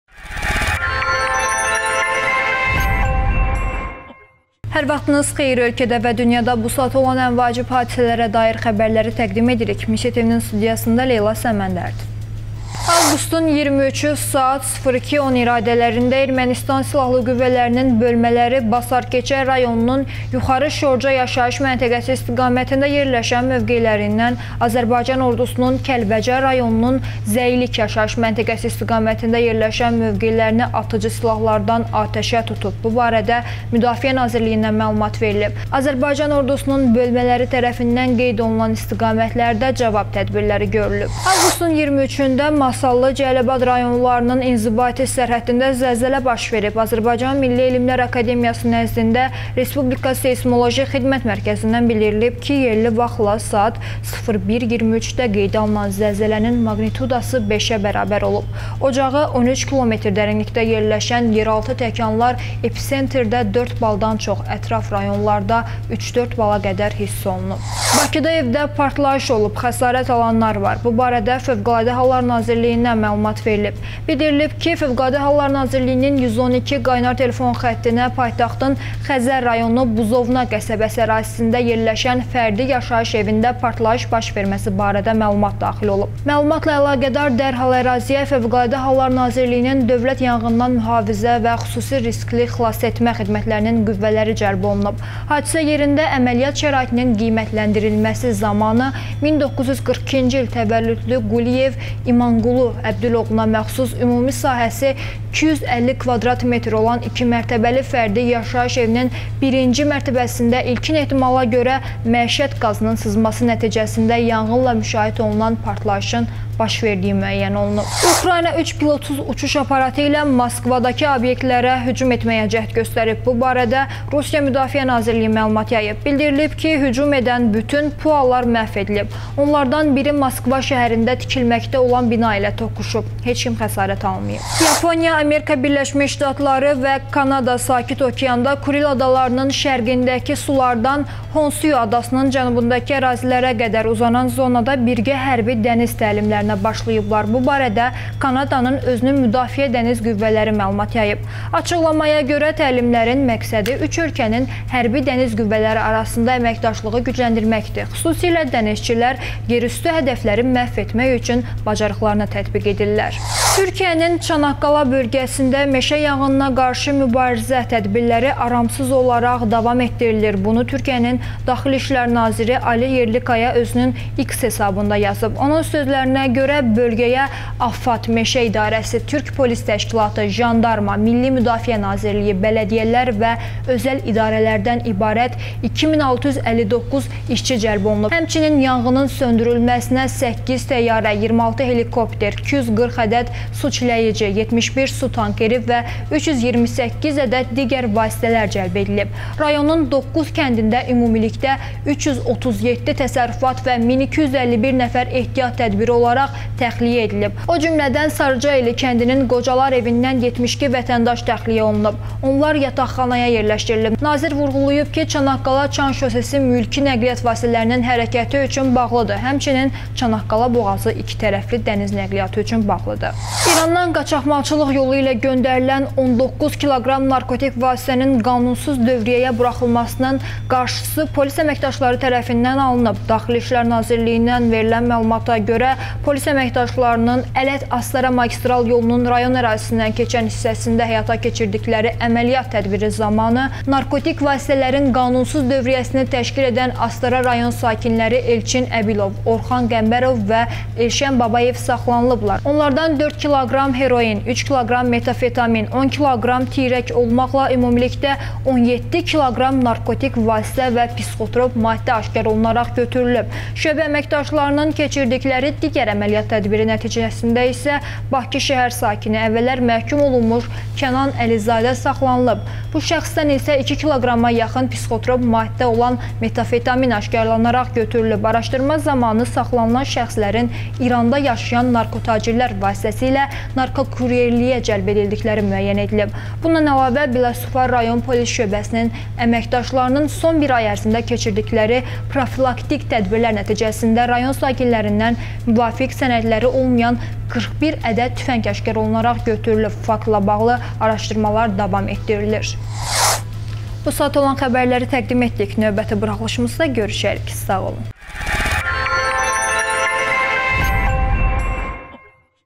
🔴 Bakıda evdə partlayış olub, xəsarət alanlar var
✅ Ermənistan silahlı bölmələri Azərbaycan Ordusunun Kəlbəcərdəki mövqelərini atəşə tutub
✅ Azərbaycanda zəlzələ olub
✅ Moskvaya dron hücumunun qarşısı alınıb
✅ Yaponiya, ABŞ və Kanada Kuril adlarında dəniz təlimlərinə başlayıblar
✅ Çanaqqalada şiddətli meşə yanğını davam edir: 8 təyyarə, 26 helikopter cəlb edilib
✅ Biləsuvarda saxlanılan narkokuryerlərdən 19 kiloqram narkotik vasitə aşkarlanıb